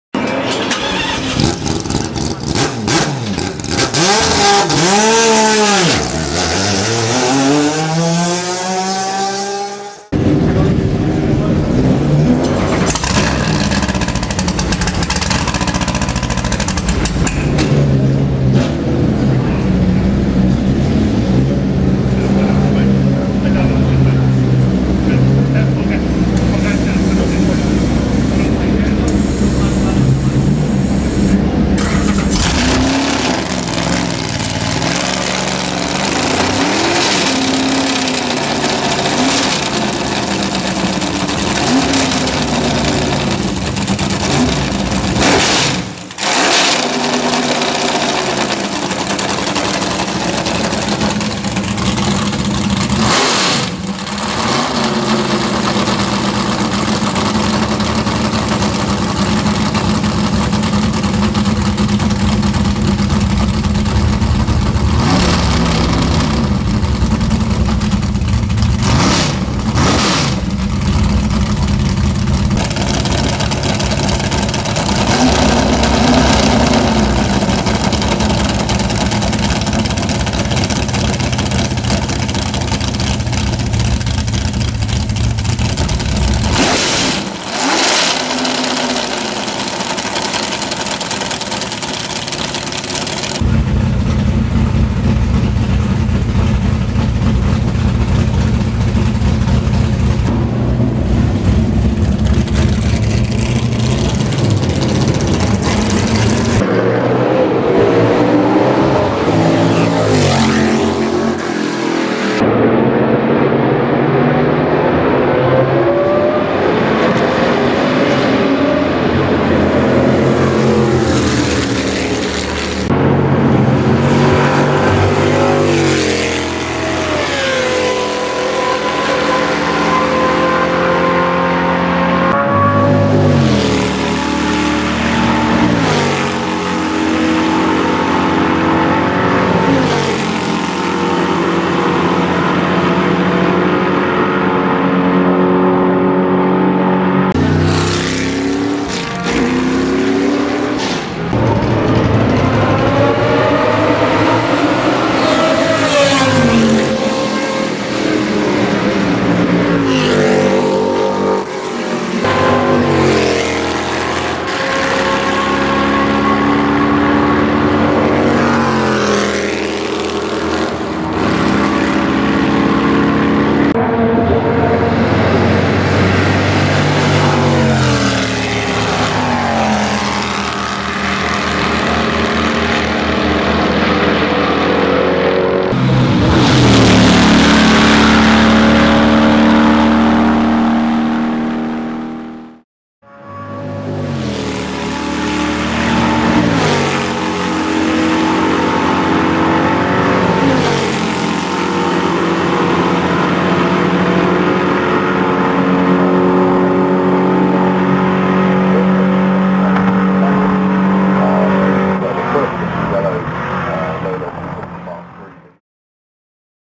1971 March 717 Canam at Spa 2013 (very loud revving) 8,8L 800hp
Engine: Chevrolet 90º V8 8800 cc / 537 cu in / Naturally Aspirated
Power: 800 bhp / 597 KW
Weight: 800 kilo / 1763.7 lbs
Top Speed: 325 km/h (202 mph)

event: Spa 6 hours
race series: CanAm Interserie Challenge
date: 20th and 21st of september 2013

driver: Frank Bradley (GB)
qualified 4th overall from 21 entries and finished 4th from 15 finishers.

Filmed with 2 different camera's:
Canon Legria HF M46
Canon EOS 7D + Røde Videomic Pro